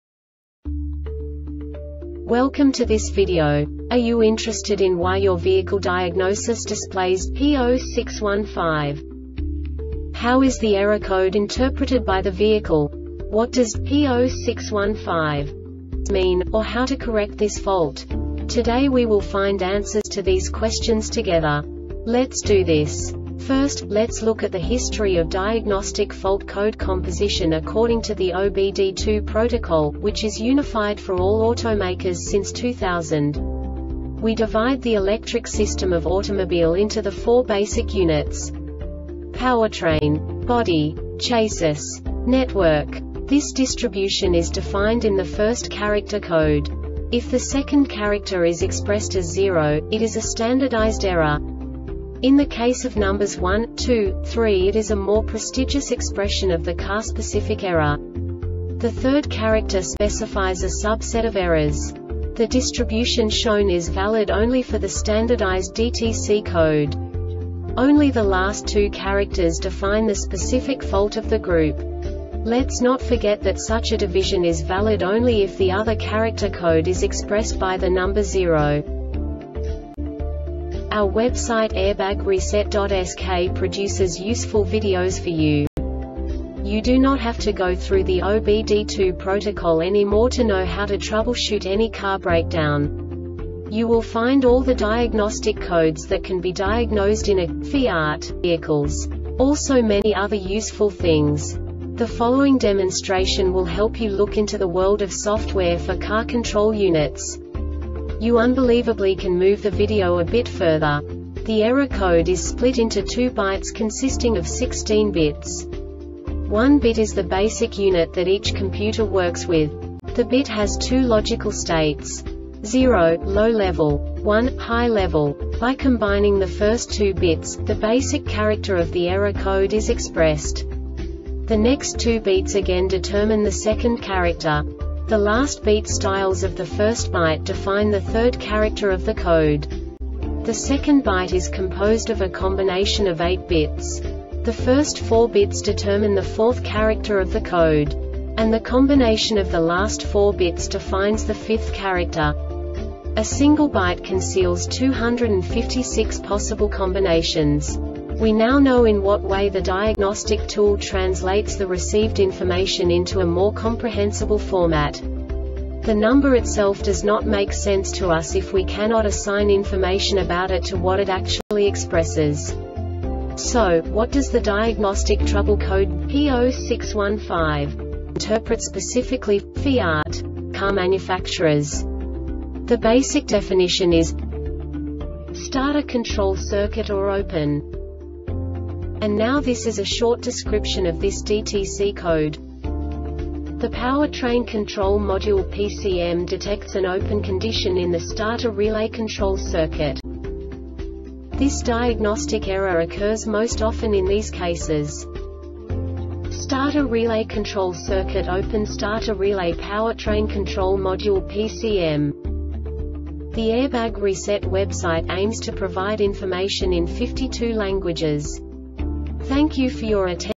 DTC Fiat P0615 Short Explanation
Contents:
0:21 Basic DTC analysis according to OBD2 protocol standard.
1:48 Insight into programming
2:58 A diagnostically understandable description of the Diagnostic Trouble Code
3:05 Basic error definition

Make:
FIAT
Code:
P0615
Definition:
Starter Control Circuit or Open
Description:
The Powertrain Control Module (PCM) detects an open condition in the starter relay control circuit.
Cause:
Starter Relay Control Circuit Open
Starter Relay